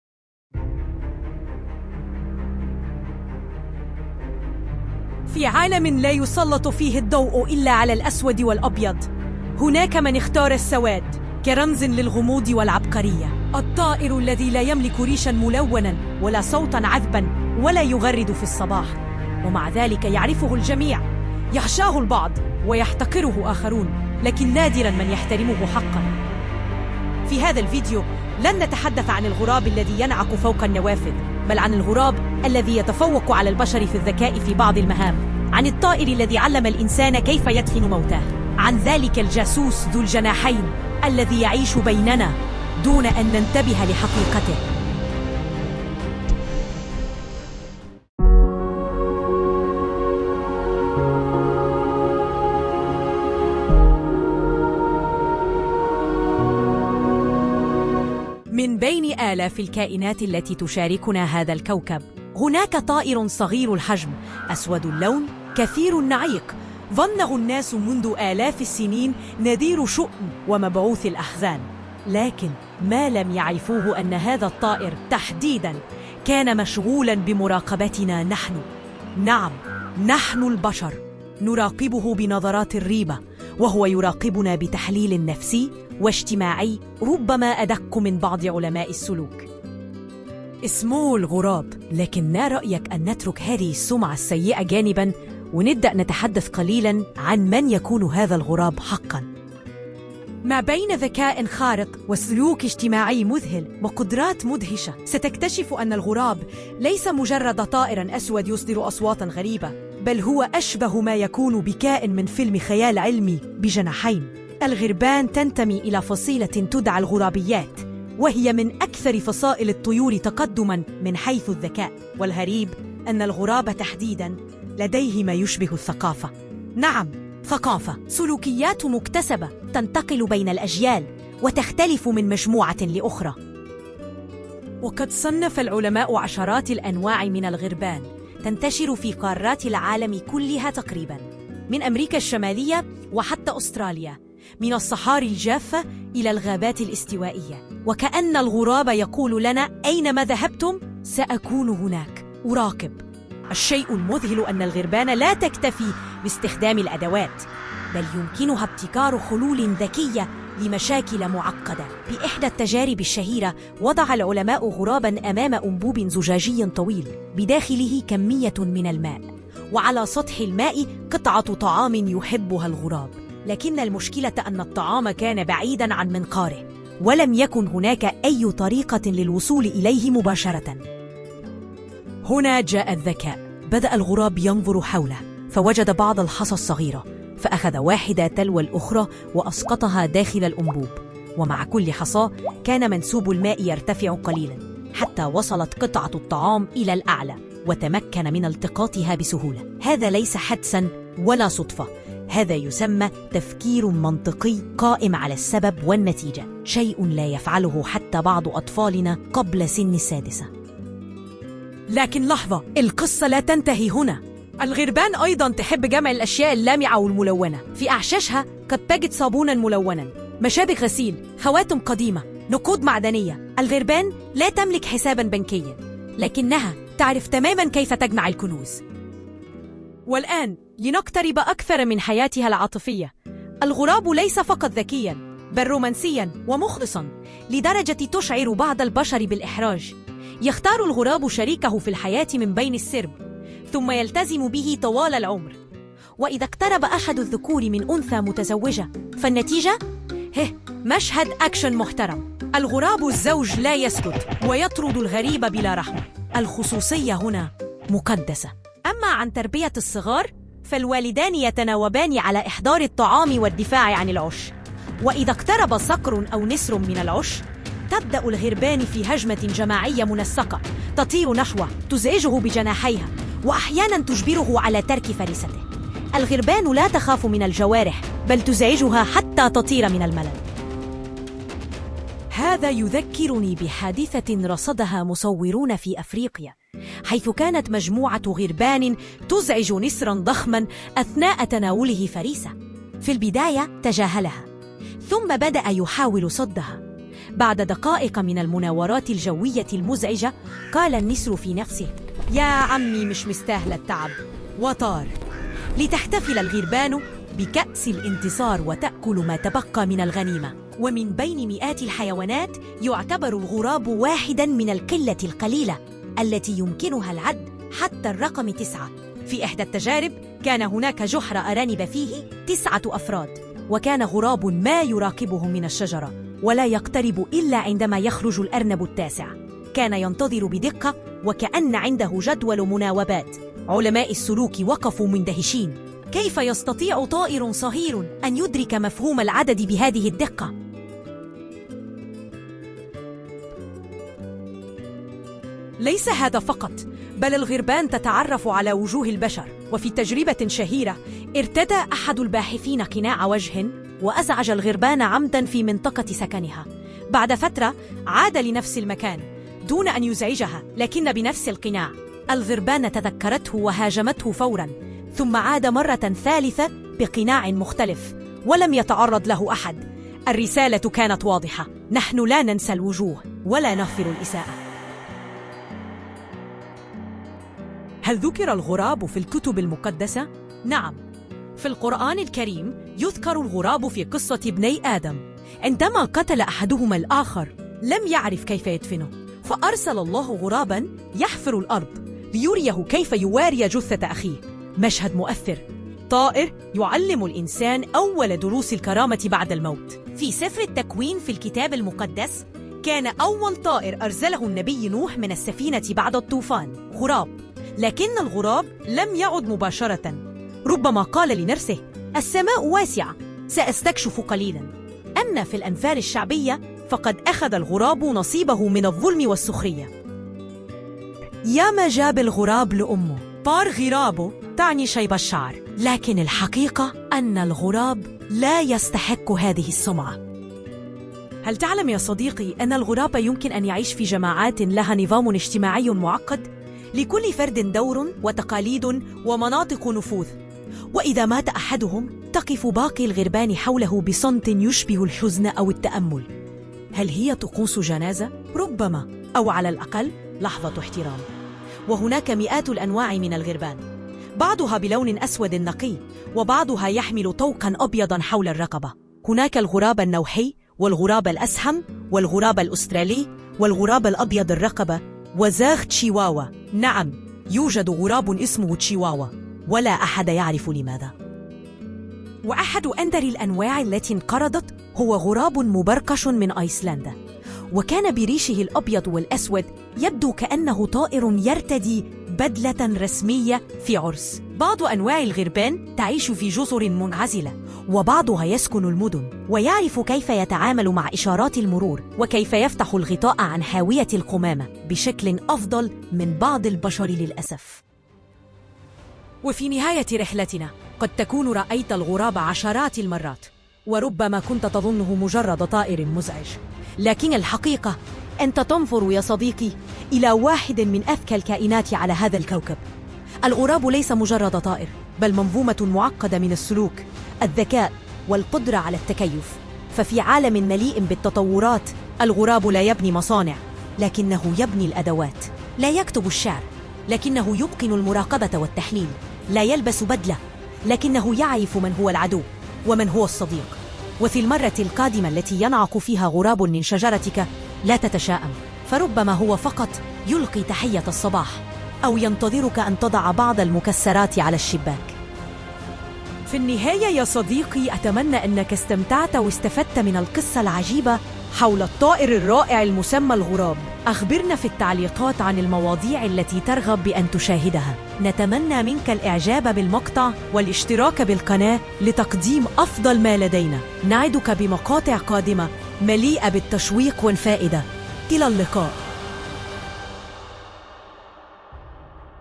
من طائر مشؤوم إلى معجزة ذكية: قصة الغراب
في هذا الفيديو، نكشف الوجه الحقيقي لأحد أكثر الطيور غموضًا وسوء فهم: الغراب.
ليس مجرد طائر أسود ينعق، بل مخلوق مذهل بذكاء خارق، سلوك اجتماعي معقد، وذكريات لا تُنسى!
سنروي لك أغرب القصص والتجارب العلمية حول الغربان، ونتأمل في ظهورها في الكتب المقدسة، وثقافات الشعوب.

🚨 شاهد الفيديو للنهاية... وتعرّف على الطائر الذي لا يجب أن تكرهه بعد اليوم!